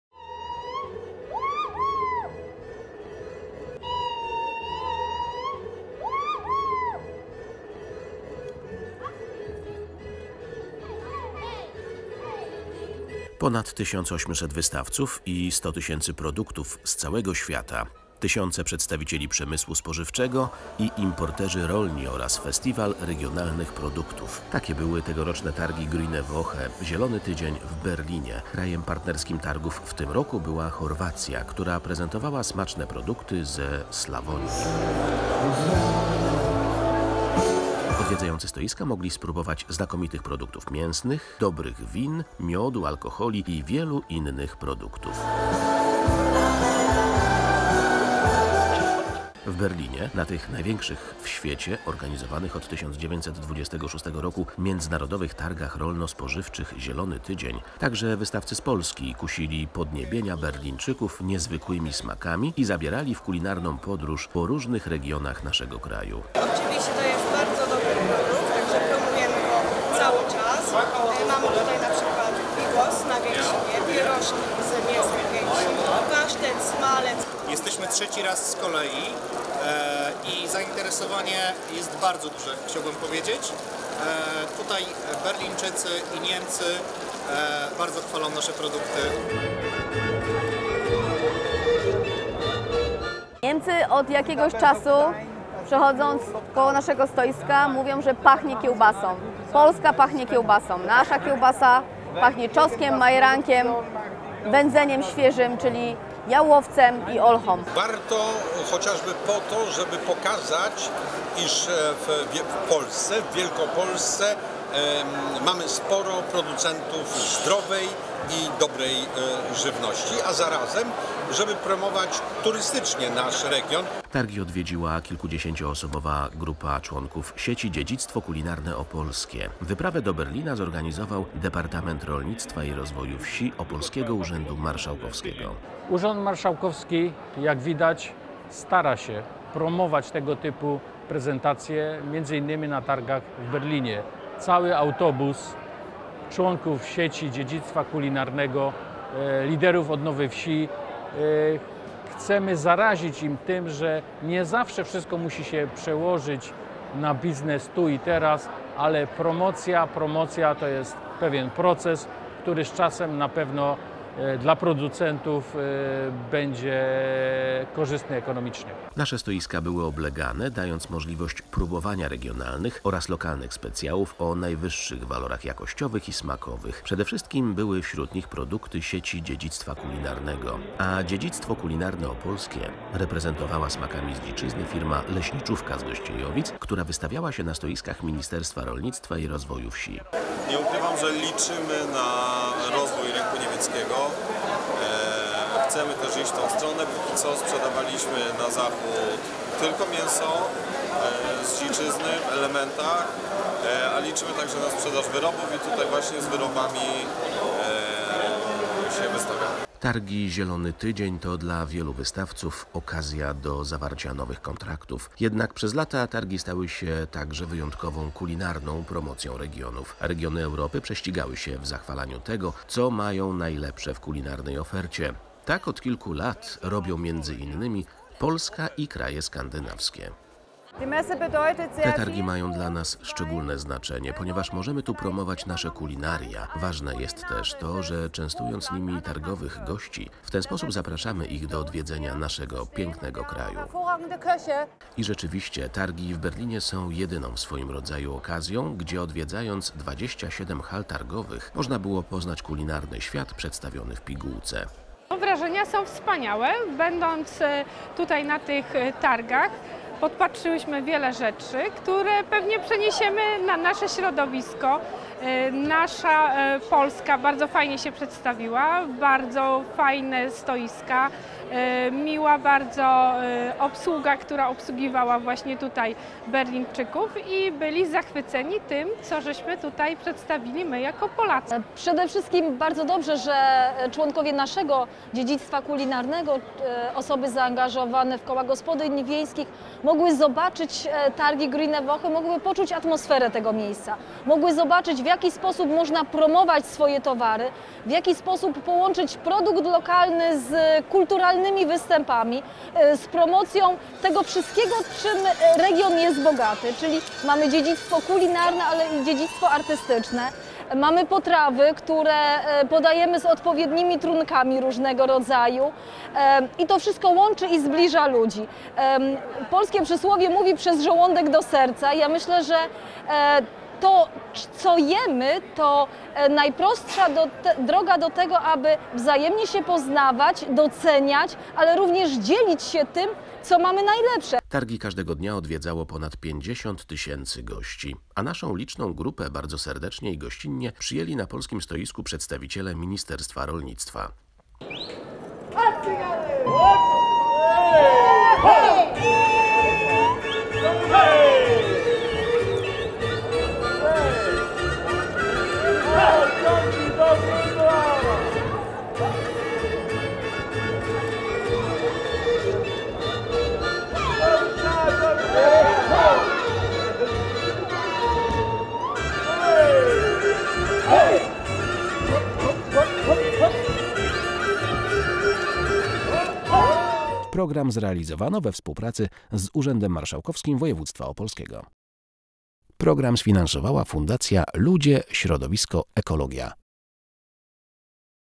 INTERNATIONALE GRÜNE WOCHE BERLIN 2020
Ponad 1800 wystawców i 100 000 produktów z całego świata. Tysiące przedstawicieli przemysłu spożywczego i importerzy rolni oraz festiwal regionalnych produktów. Takie były tegoroczne targi Grüne Woche - Zielony Tydzień w Berlinie. Krajem partnerskim Tragów w tym roku była Chorwacja, która prezentowała smaczne produkty ze Slawonii. Odwiedzający stoiska mogli spróbować znakomitych produktów mięsnych, dobrych win, miodu, alkoholi i wielu innych produktów.